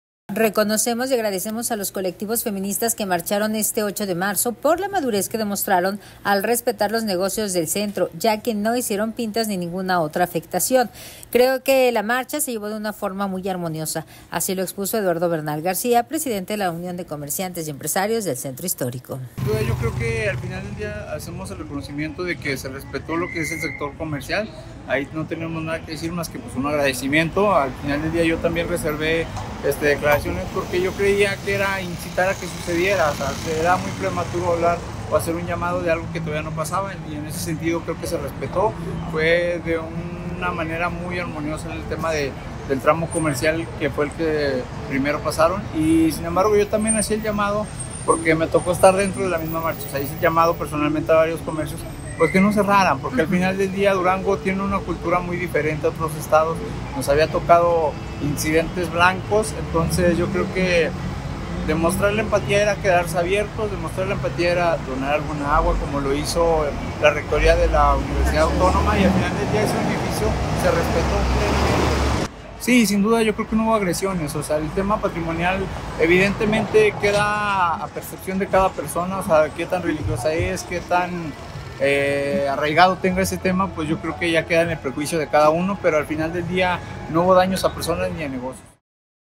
Comerciantes agradecen a feministas que respetaron sus negocios
Reconocemos y agradecemos a los colectivos feministas que marcharon este 8M por la madurez que demostraron al respetar los negocios del centro ya que no hicieron pintas ni ninguna otra afectación. Creo que la marcha se llevó de una forma muy armoniosa, expuso Eduardo Bernal García, presidente de la Unión de Comerciantes y Empresarios del Centro Histórico.